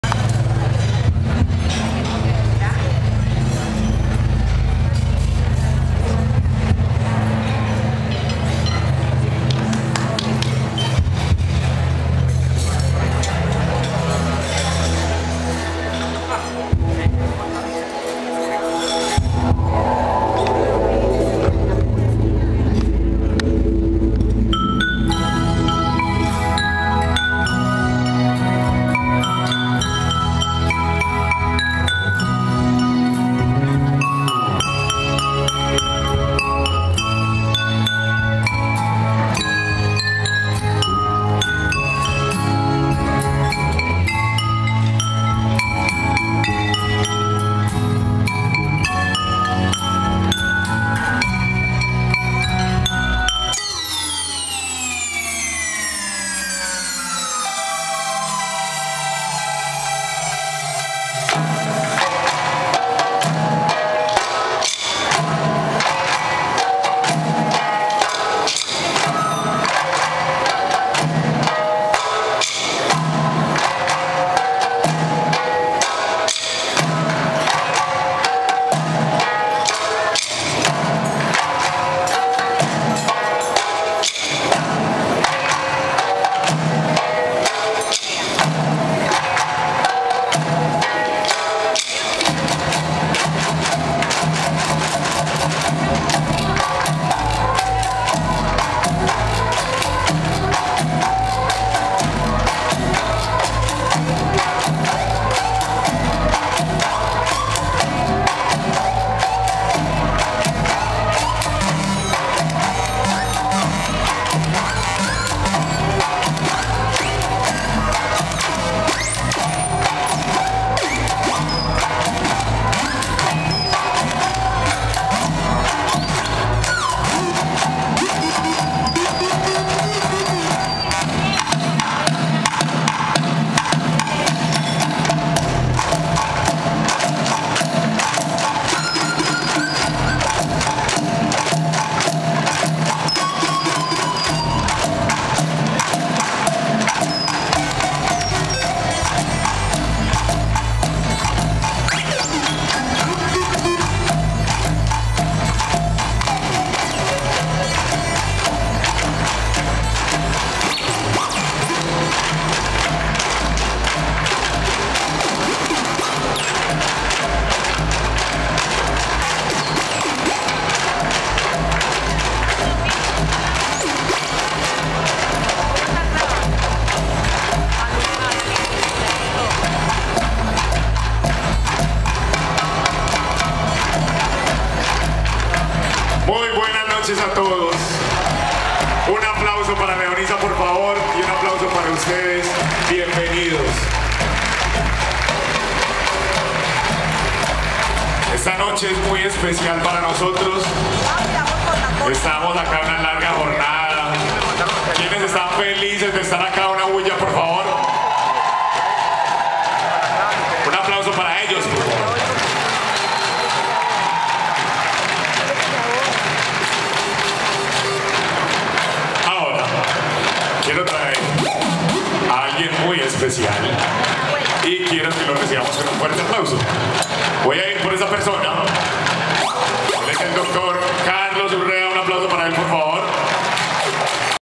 Show Leonisa - Puntacana (Rep. Dominicana) parte 1
Show a cargo de los gerentes de ventas regionales Latam de Leonisa, dirigidos por Mauricio Estrada, con la asistencia de Pedro Rincón y Maria Clara Mejía después de un taller empresarial de percusión experimental. Ellos tuvieron tres sesiones y lograron montar este espectáculo!